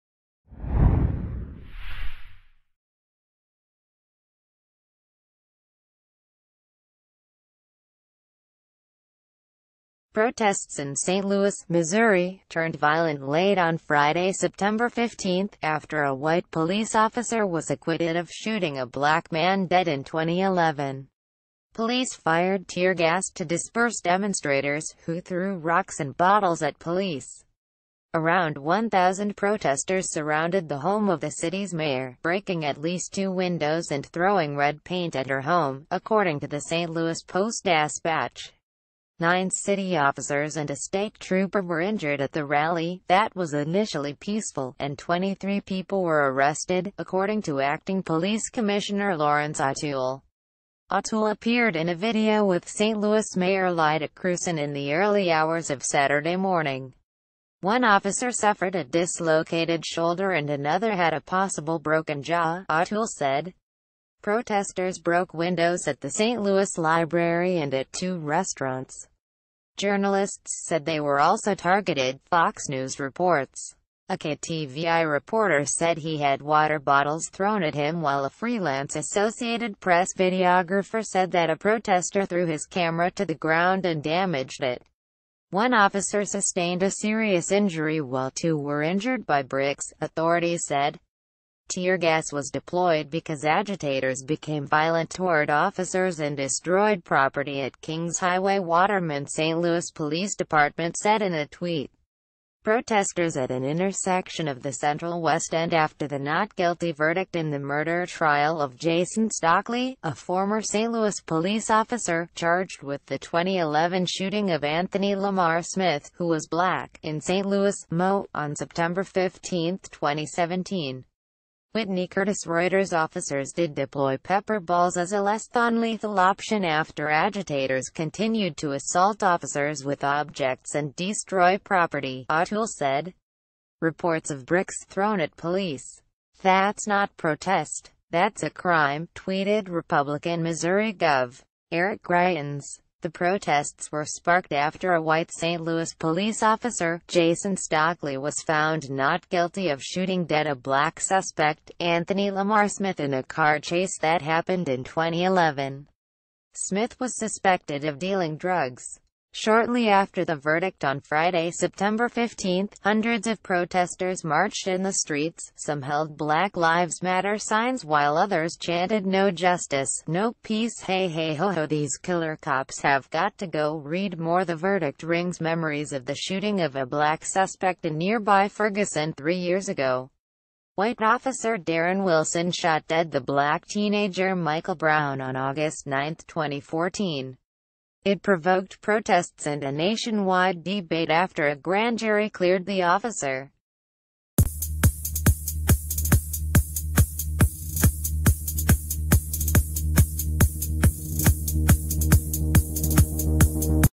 [ HOT NEW ]Protests in st louis turn violent
Protests in st louis turn violent
Protests in St. Louis, Missouri, turned violent la...